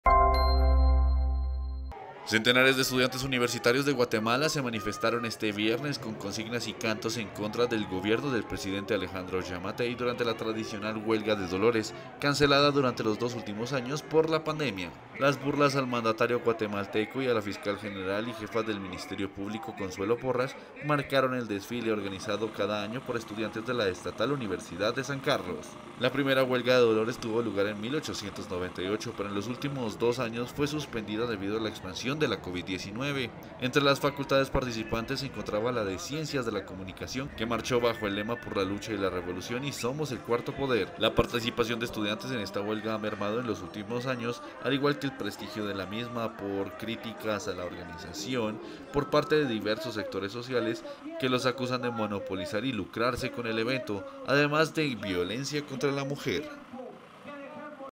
Estudiantes de la Universidad San Carlos en Guatemala realizan la tradicional Huelga de Dolores
Cada Viernes de Dolores los estudiantes sancarlistas marchan por las calles del centro histórico de la ciudad y protestan contra el gobierno de turno, este año las burlas fueron dirigidas al presidente de Guatemala Alejandro Giammattei y a la fiscal general Consuelo Porras.

¡Visítenos!